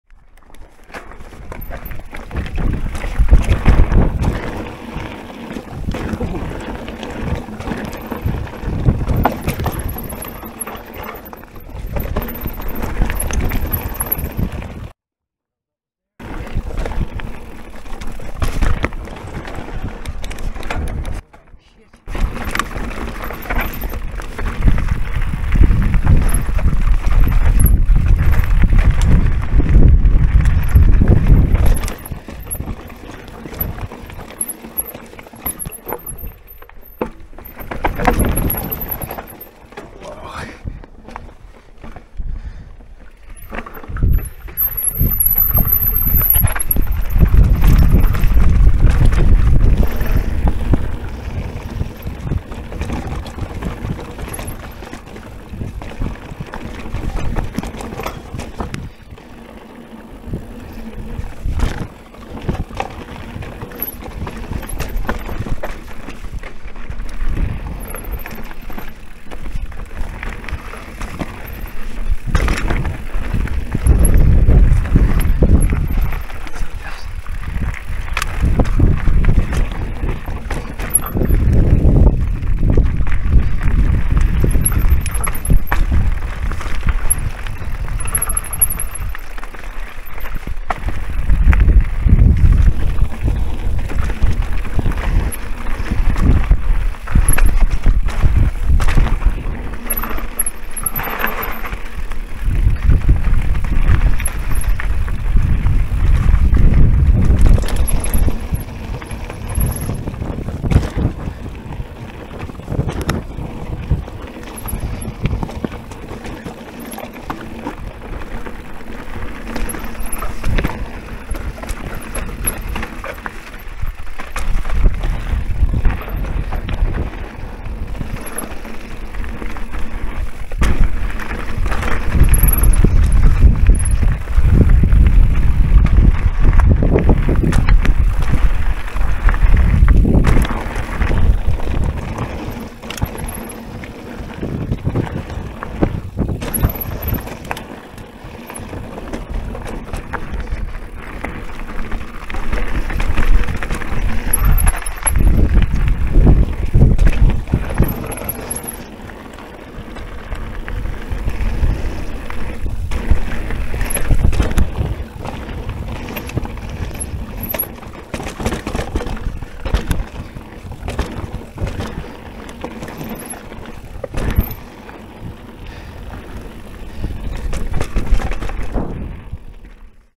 Ballinastoe, New Peaty Trail ***NEW TRAIL***
Ballinastoe,
New Peaty Trail,
Black Trail 1.13km long,
Moustache Ebike,
Dji Action 3 Camera.